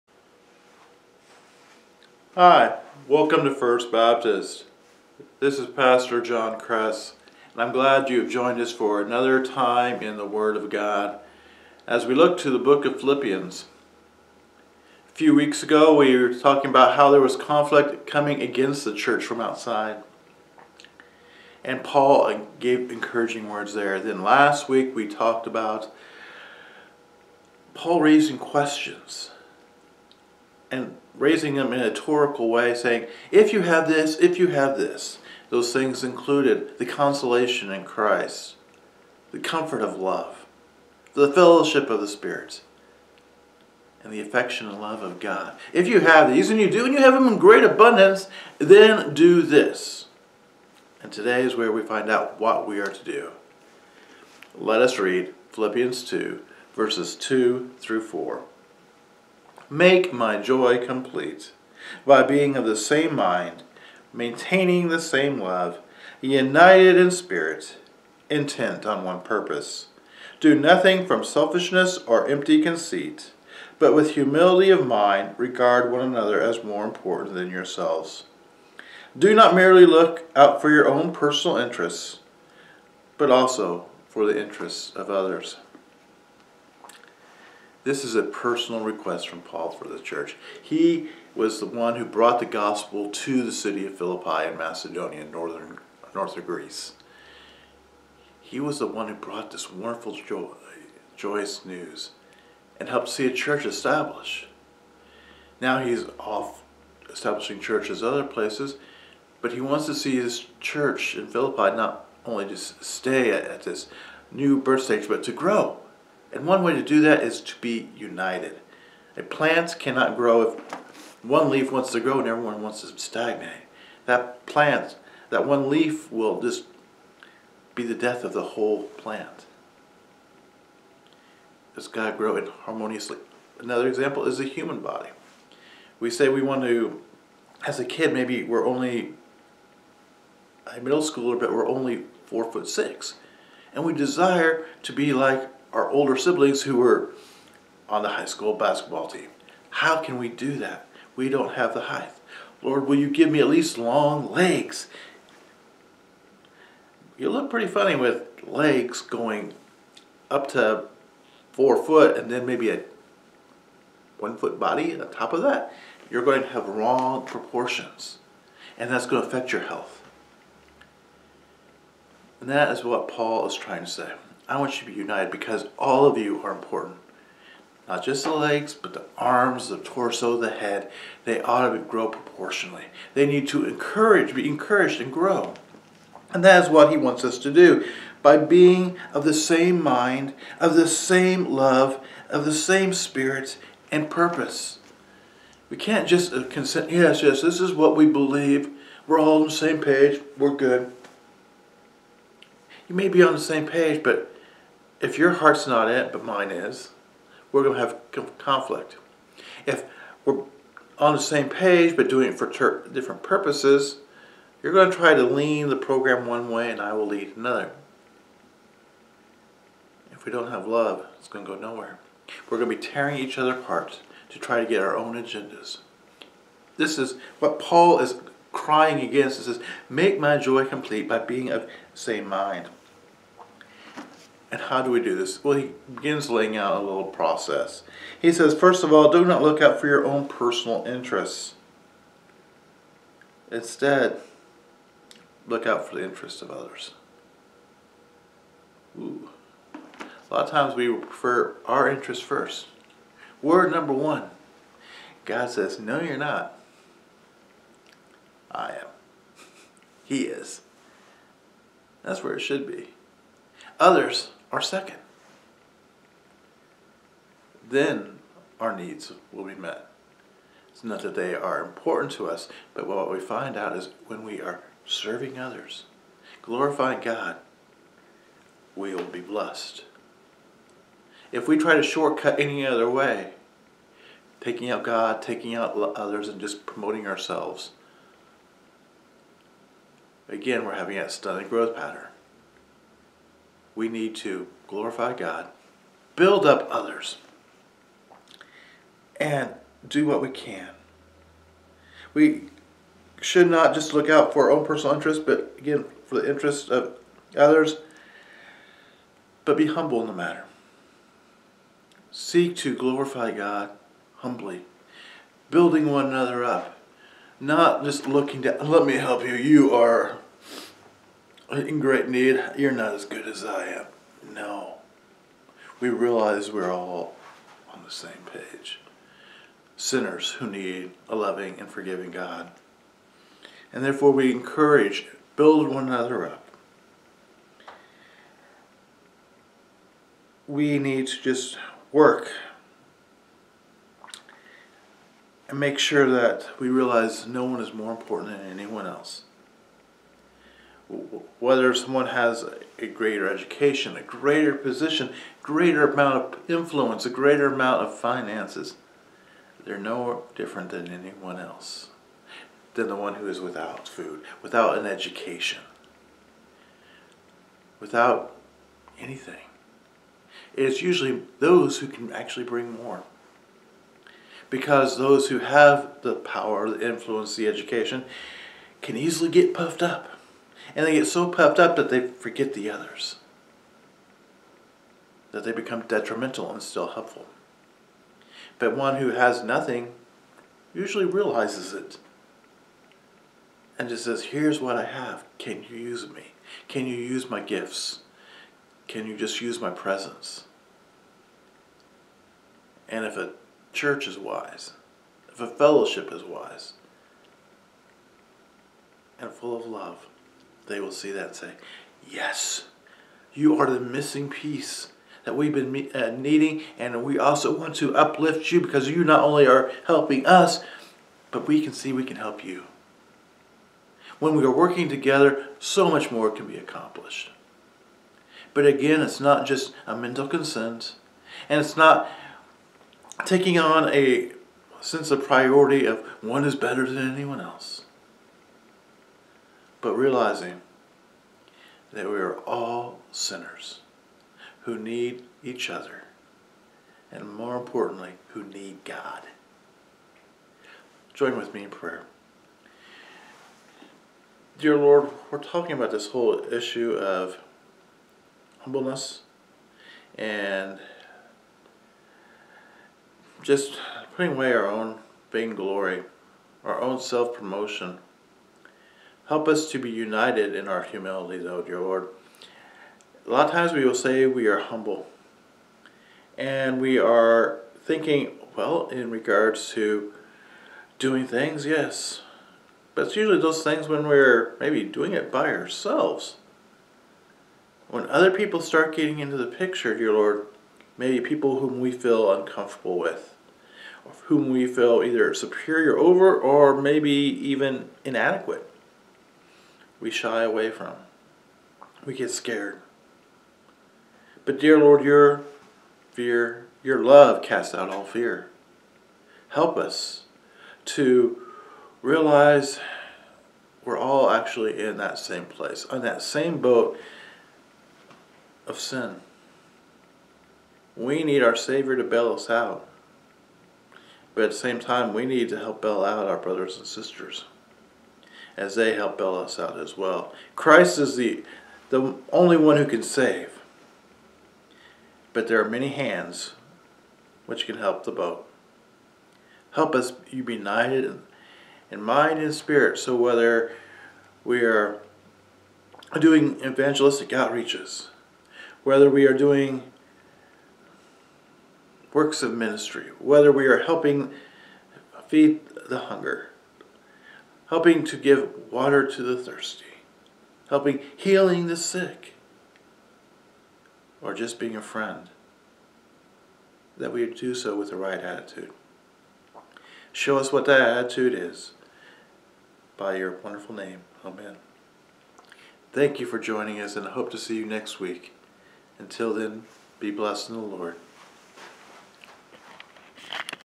22 02 02 Philippians 02 02 04 Devotion - First Baptist Horton KS
Pastor John Kress discusses from Philippians 2:2-4 how unity is achieved through lack of conceit and selfishness, but looking out the interest of others as well.